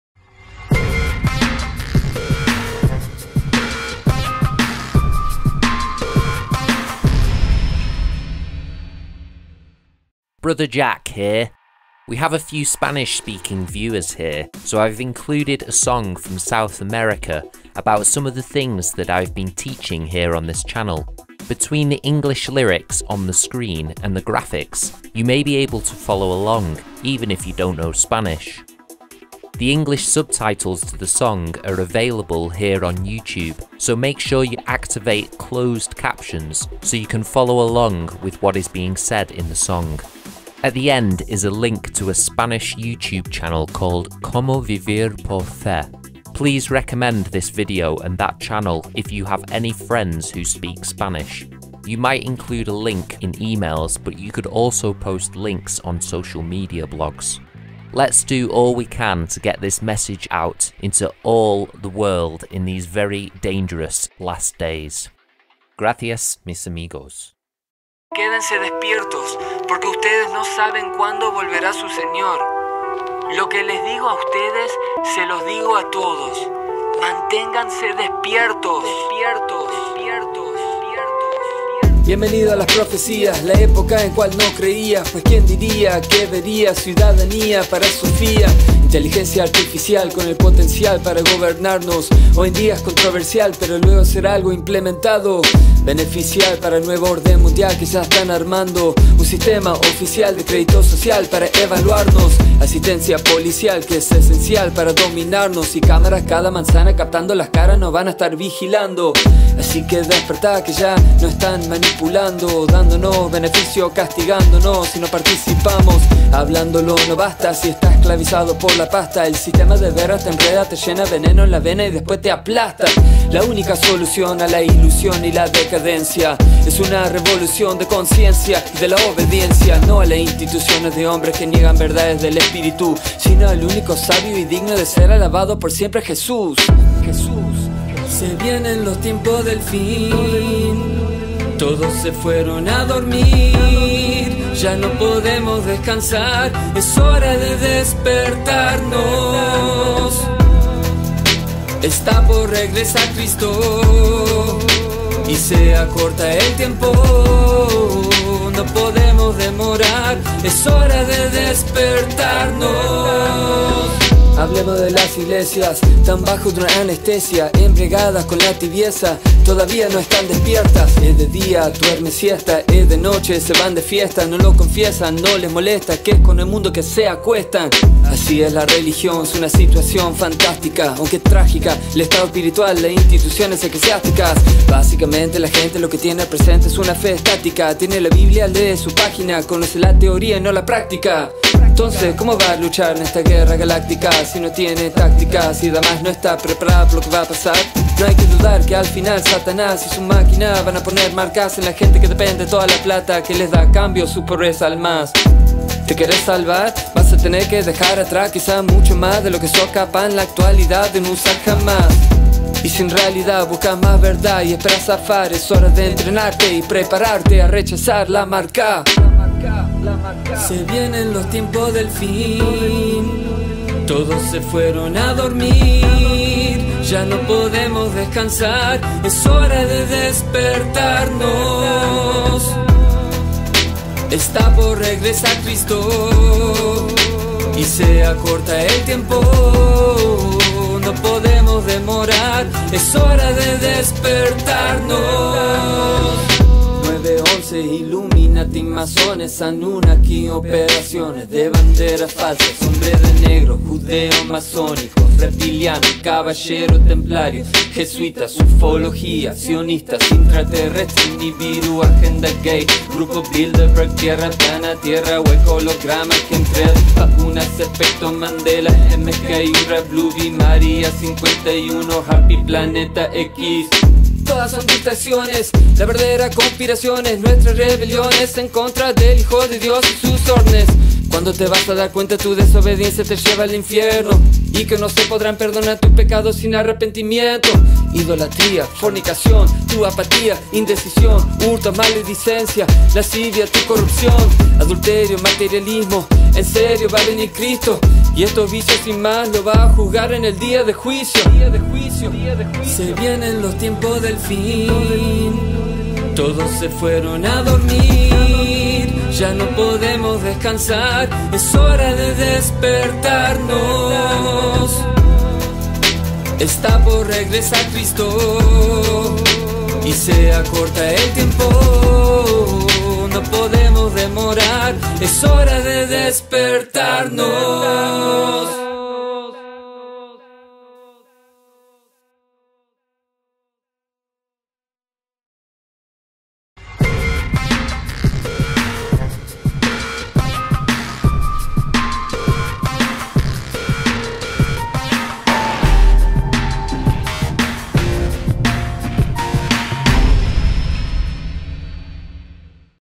End Time Song about the Mark of the Beast!
This prophetic song about the end times and the Mark of the Beast puts together some of the most relevant end time topics, such as artificial intelligence, social credit systems, and the lukewarmness in the institutional mainstream churches, as well as a warning to not get distracted by conspiracy theories.

Make sure you turn on the English subtitles for this video so you can follow along with what is being said in Spanish.